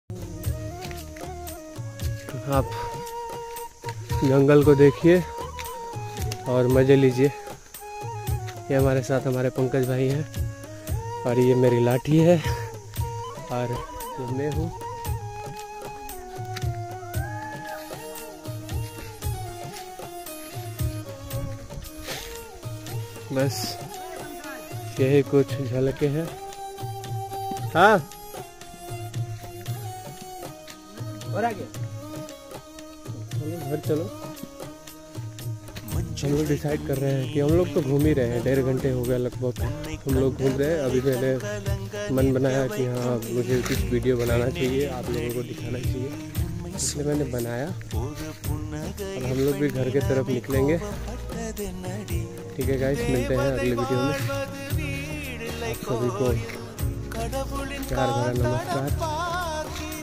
Jungle Safari in Buxa Tiger Reserve ,Buxa National Park West Bengal
jungle safari